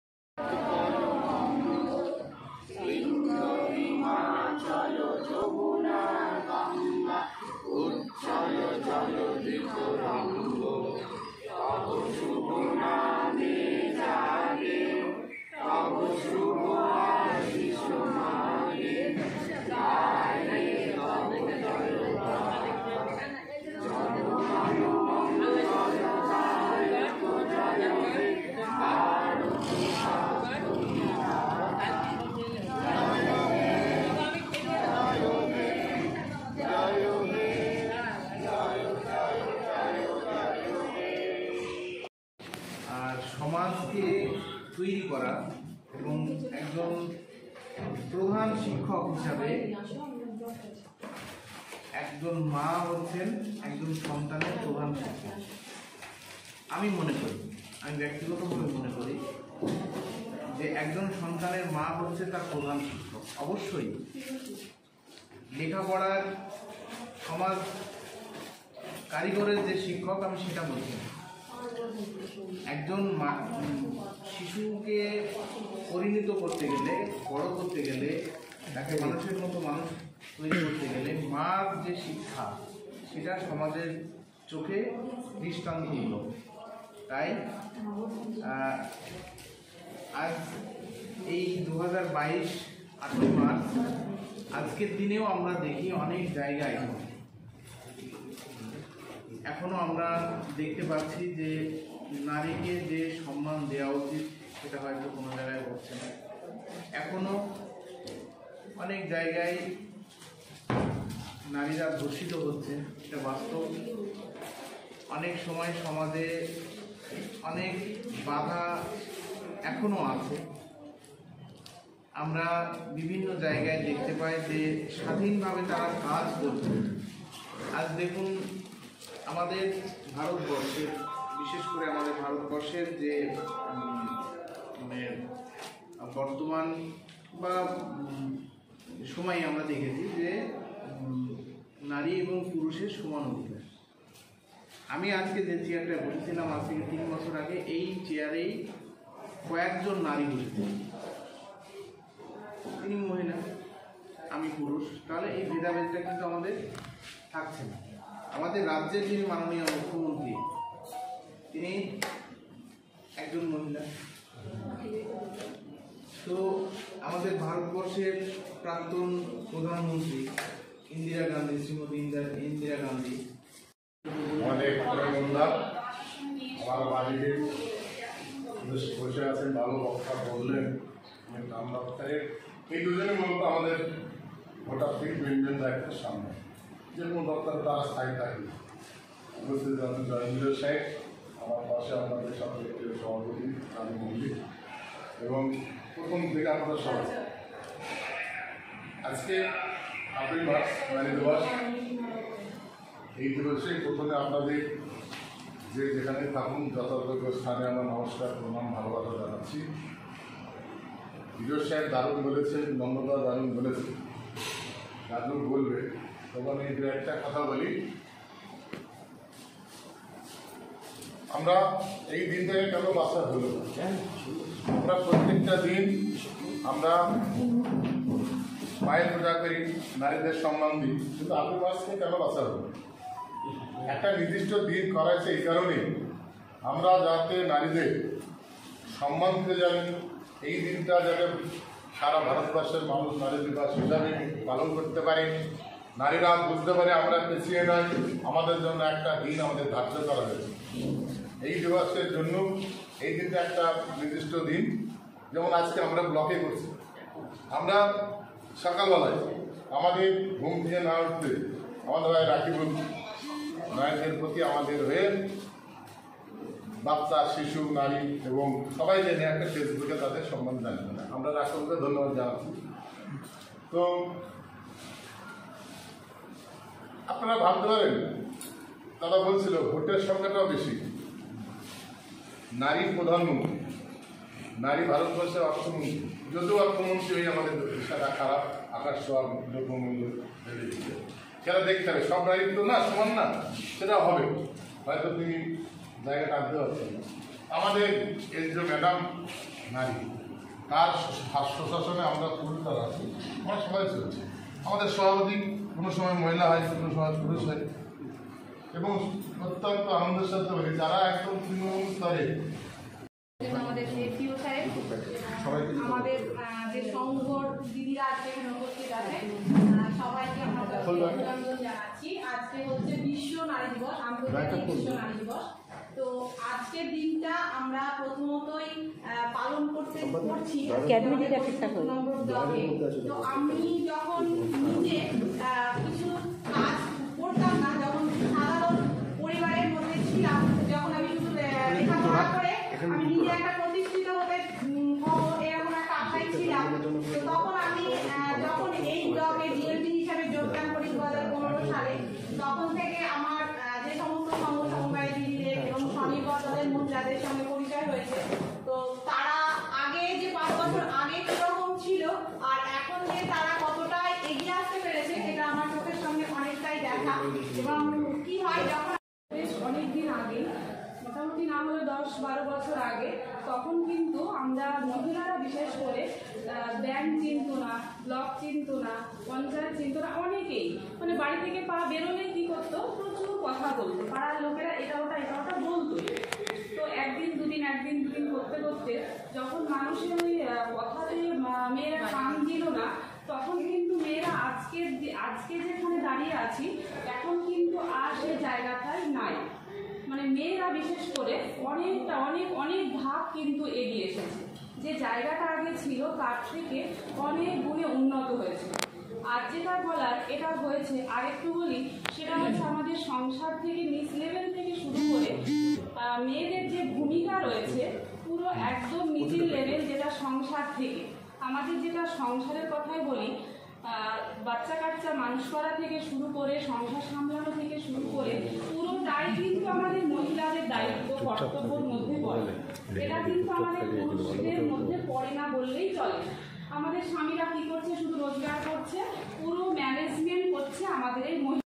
International Women's Day is being celebrated at Karimpur-2-Block Development Office of Nadia
Rajya Sambad: A report from Karimpur-2-Block Development Office under Karimpur police  station area of Nadia district in West Bengal in India on 08-03-22.

 International Women's Day is being celebrated at Karimpur- 2-Block Development Office of Nadia district in West Bengal on 08-03-22.